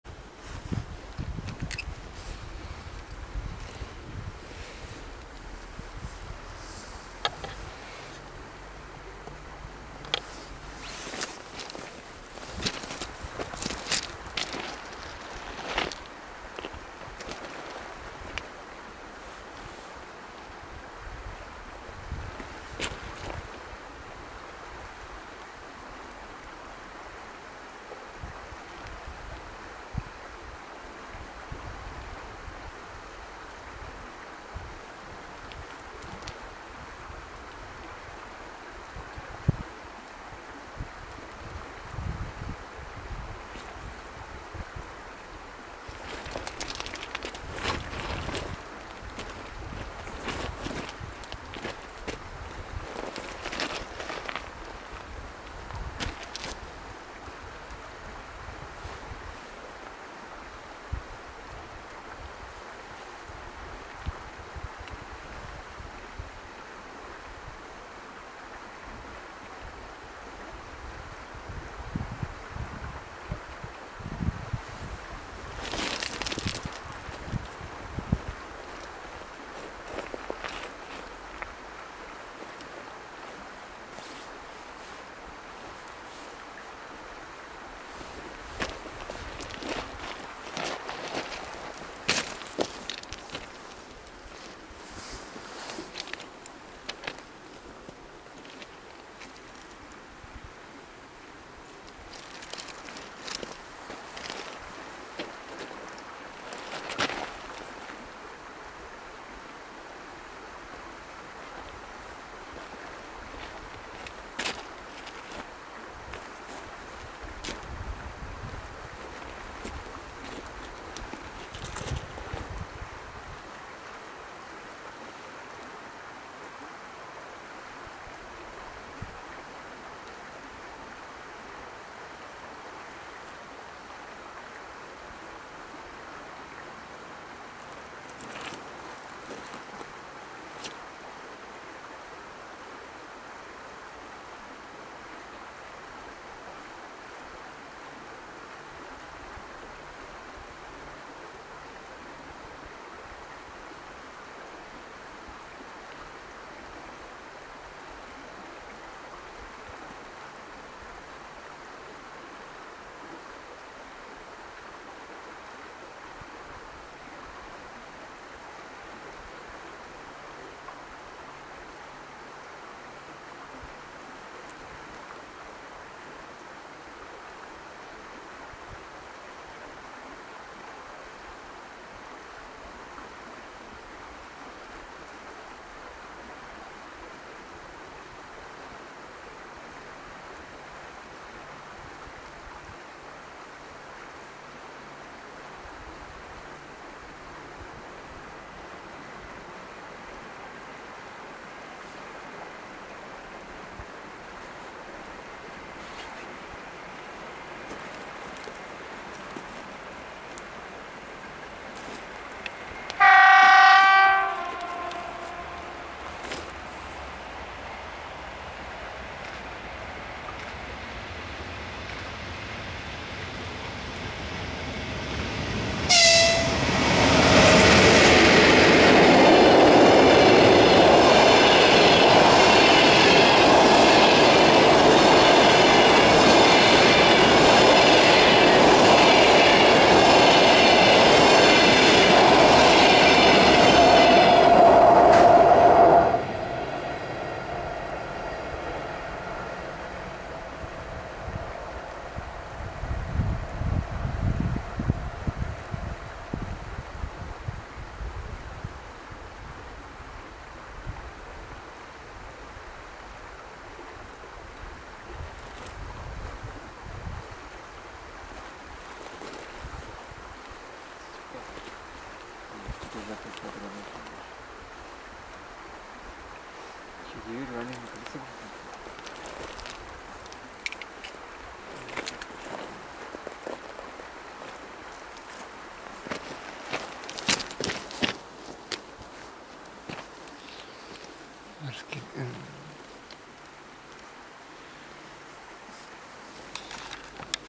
"Дождец камере" или ЭП1М-795 с дополнительным скорым поездом, перегон Агрыз-Уром
"Дождец камере" или электровоз ЭП1М-795 с дополнительным скорым поездом №231Г сообщением Ижевск - Москва, перегон Агрыз-Уром
5.11.18
Россия/Удмуртия